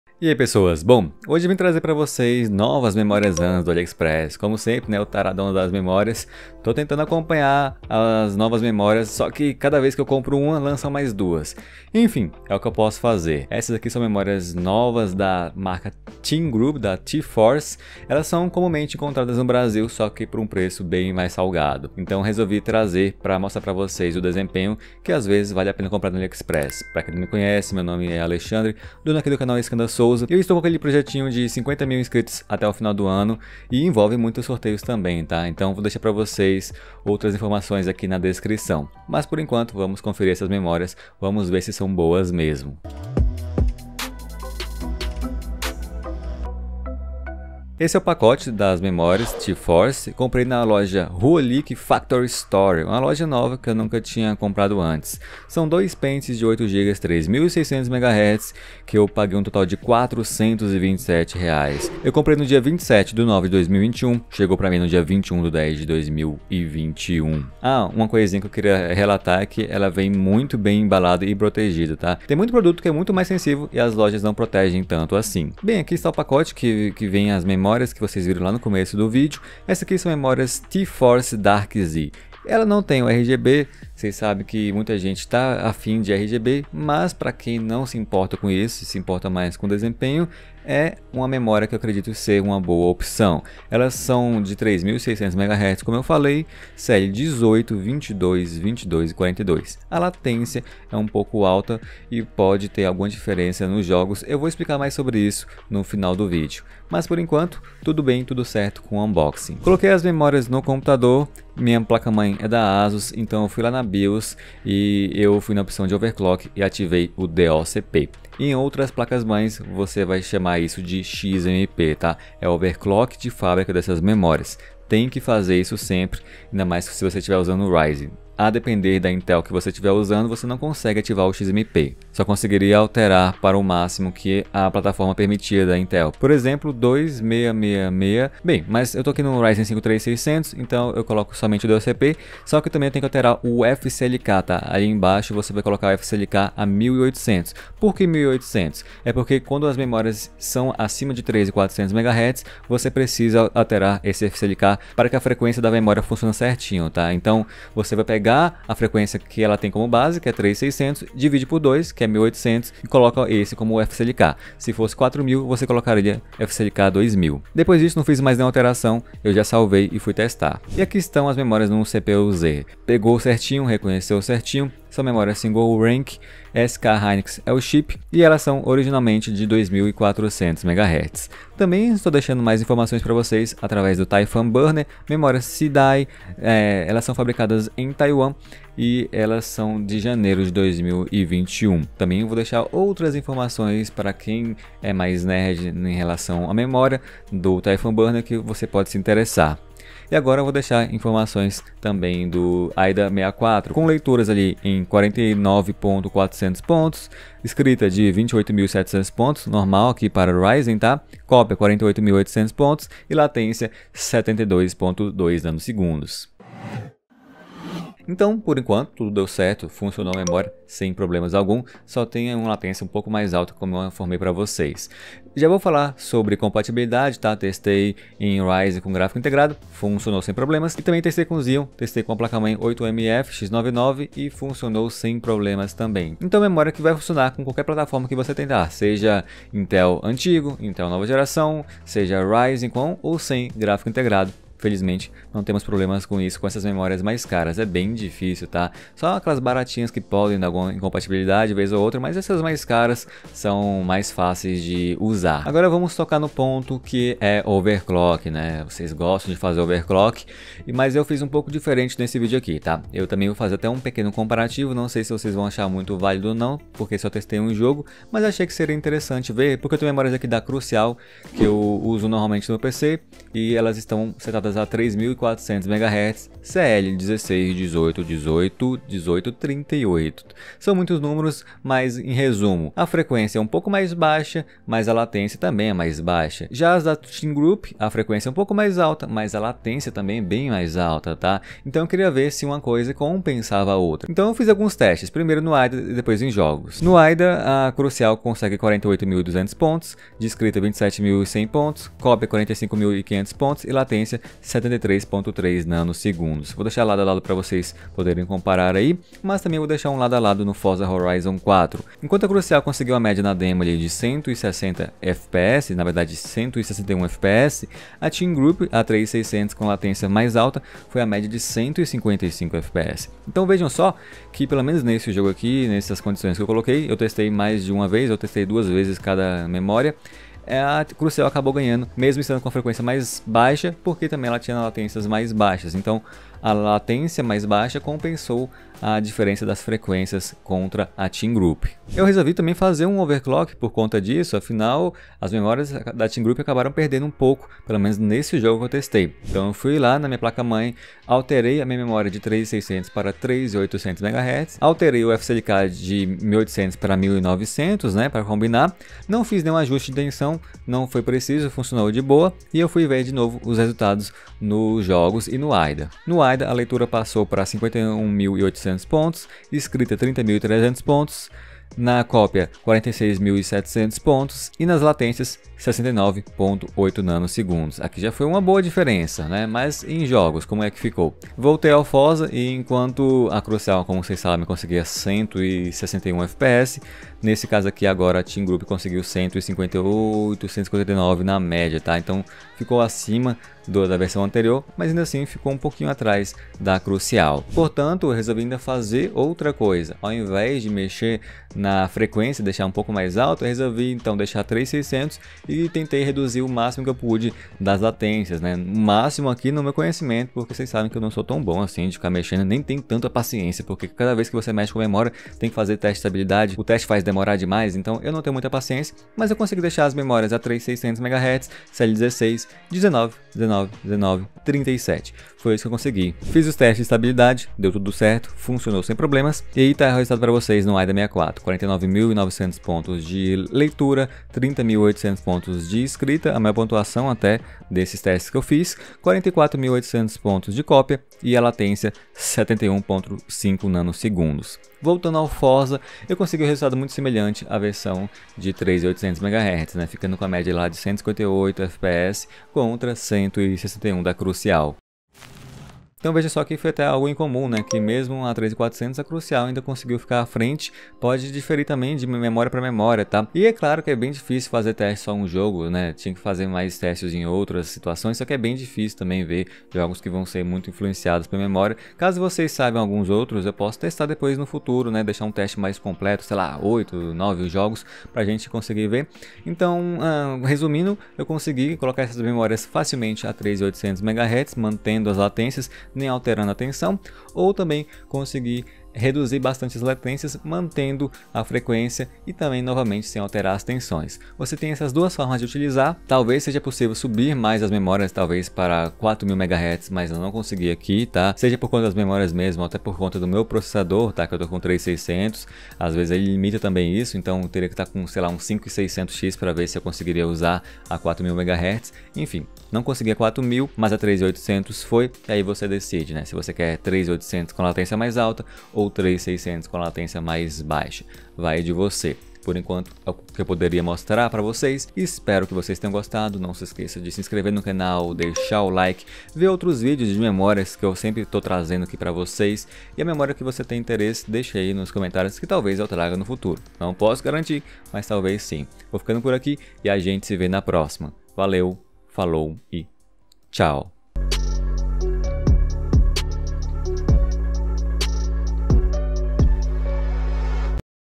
Lançamento Aliexpress! Memória DDR4 T-FORCE DARK Z 3600MHZ: MEMÓRIAS para Ryzen, Xeon ou Intel Novo
Essas são memórias ram ddr4 da TEAM GROUP. Elas já são mais comuns aqui no Brasil, mas apareceram recentemente no Aliexpress por um preço muito bom! Será que funciona em xeon?Faz overclock? São boas? é o que vamos ver!

Compra: 27/09/2021  | Entregue: 21/10/2021 | Valor: R$ 427,00

➍ Confiram a Banggood, Loja parceira do Canal
➎ Cash back (Dinheiro de volta ao comprar)
◼ Insta iskandar_souza ◼  Face: Iskandarsouza1